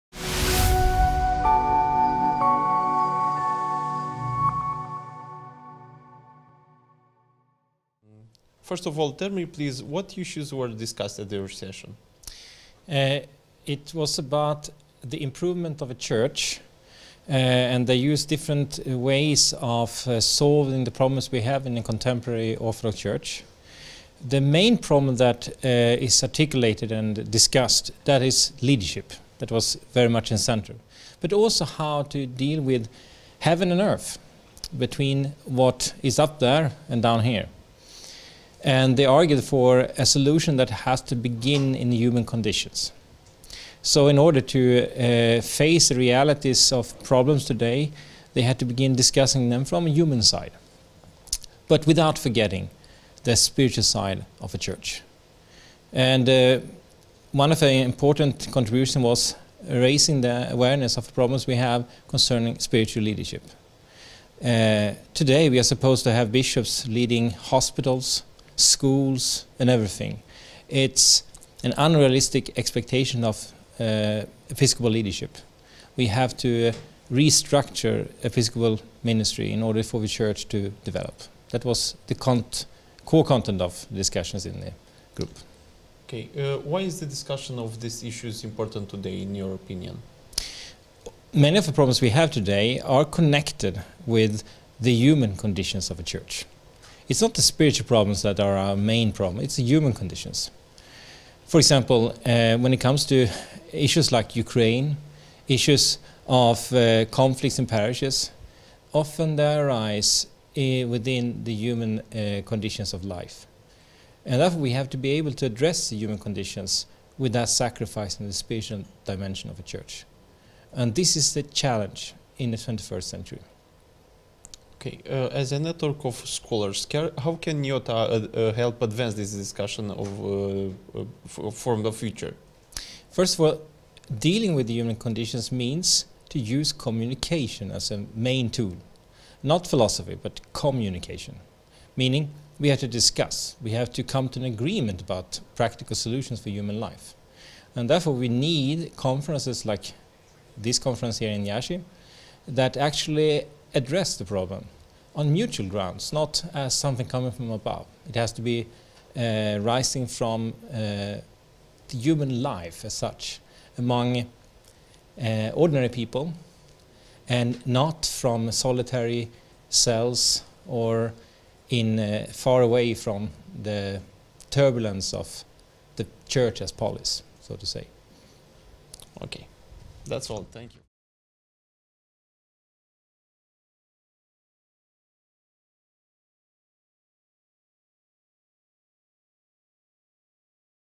Michael Hjälm on Critical Ecclesiology
Dr. Michael Hjalm, session chair, gives an overview of session 13, "Critical Ecclesiology," presented by the Ecclesiology Group at the IOTA Inaugural Conference, Iasi, Romania, 9-12 January 2019.

The session aimed at applying the method of critical analysis to both theory and practice in the church. The ultimate goal of this method is to make the self-awareness of the church sharper, more adequate, and more comprehensive for those who are inside and outside the church.